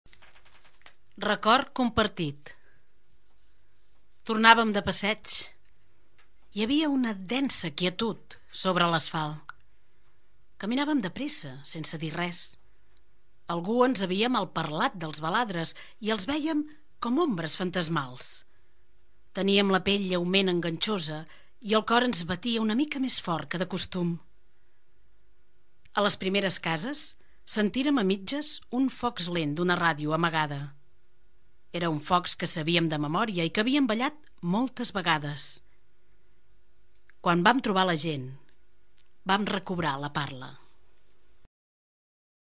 Veu i text Xavier Amorós "Record compartit"
El col·lectiu En Veu Alta posa veu al poema "Record compartit" de Xavier Amorós.